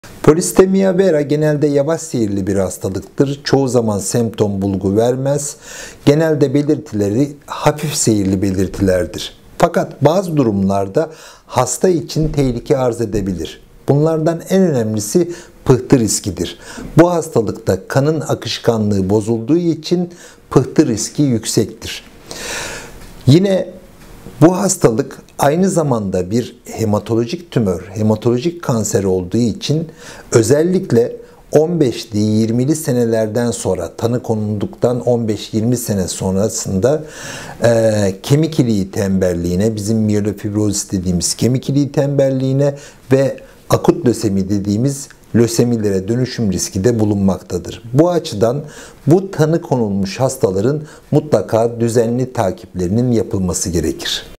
Polisitemia Vera Komplikayonları Nelerdir?
Polisitemia Vera yavaş seyirli bir hastalıktır ve genelde belirti vermemektedir.
Polisitemia Vera hastalığında pıhtı en önemli riskler arasındadır. Kanın akışkanlığı bozulduğu için pıhtı riski yüksektir.
Polisitemia Vera hematolojik kanser olduğu için özellikle tanı konulduktan 15-20 sene sonra "Myelofibrozis" adı verilen bir kemik iliği tembelliğine ve akut lösemiye dönüşme riski de bulunmaktadır.
Bu açıdan hastaların mutlaka düzenli takiplerinin yapılması gerekir.